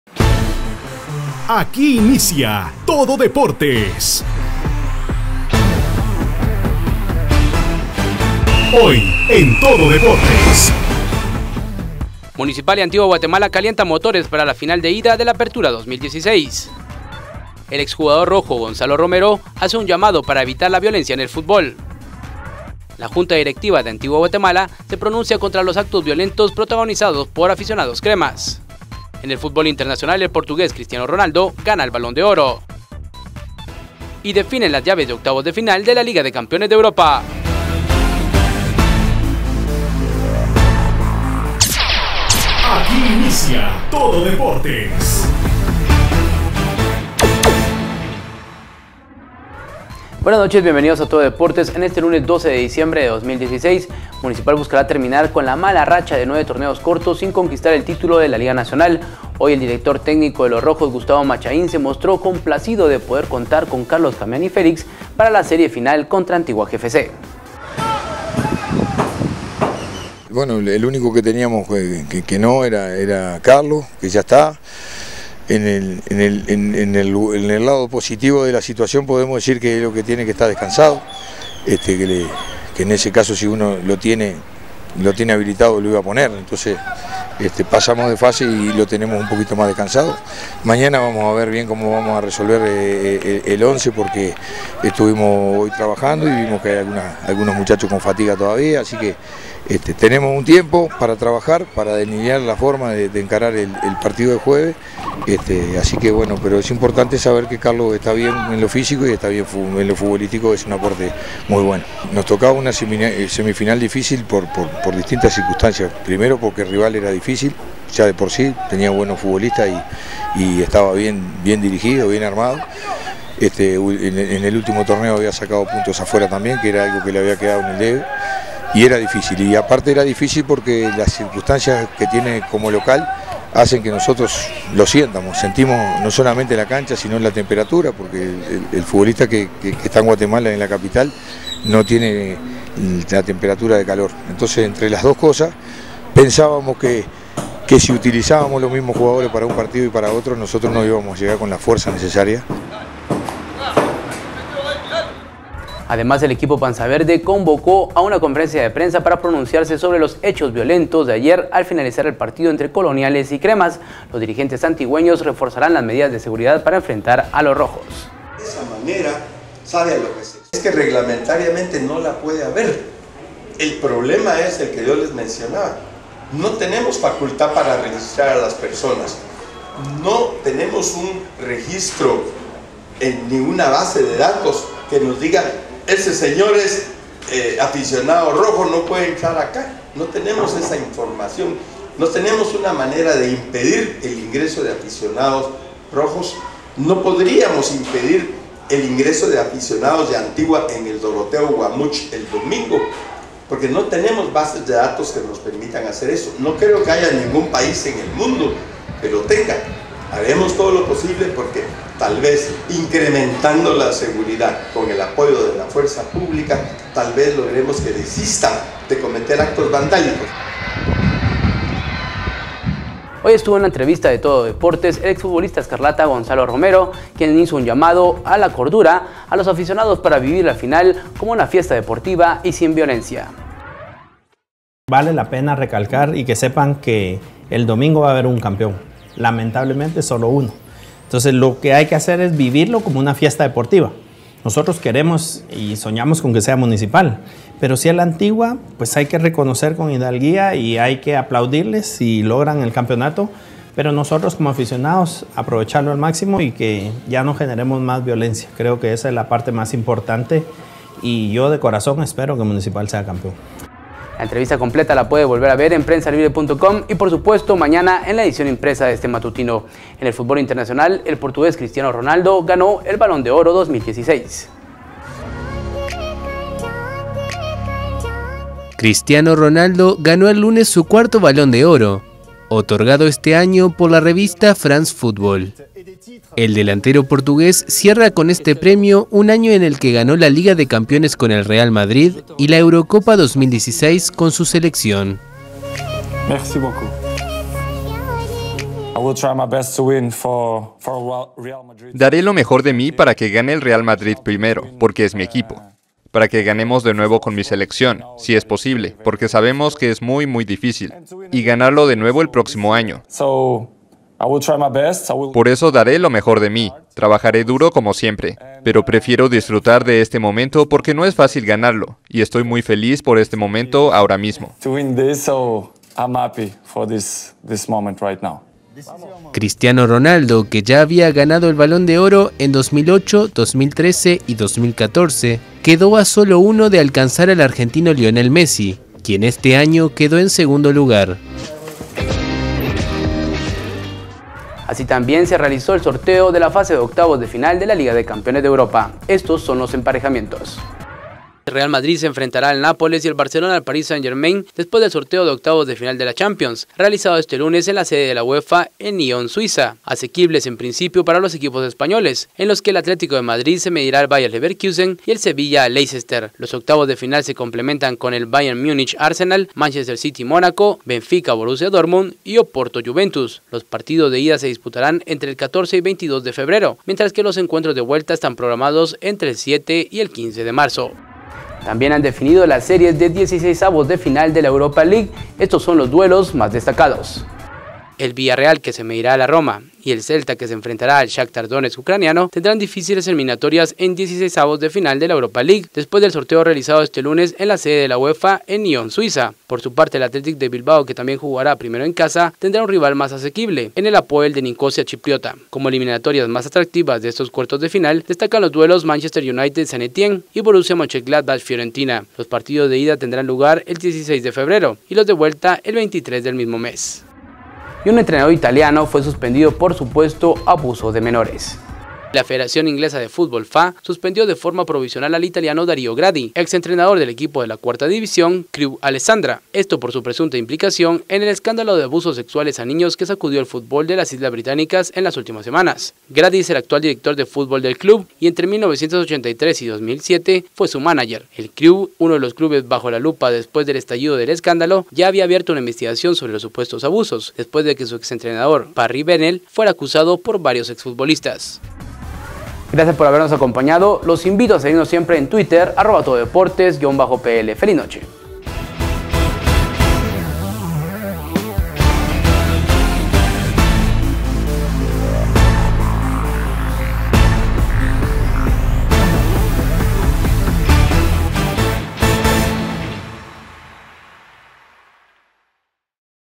Rojos y coloniales preparan el partido de ida de la final del Apertura 2016
Gustavo Machaín estudia a quienes pondrá en el once inicial para el partido de ida de la final; Antigua GFC reforzará las medidas de seguridad para evitar incidentes; Gonzalo hace un llamado contra la violencia en los estadios; Cristiano Ronaldo gana el Balón de Oro 2016; definen las llaves de octavos de final de la Liga de Campeones de Europa.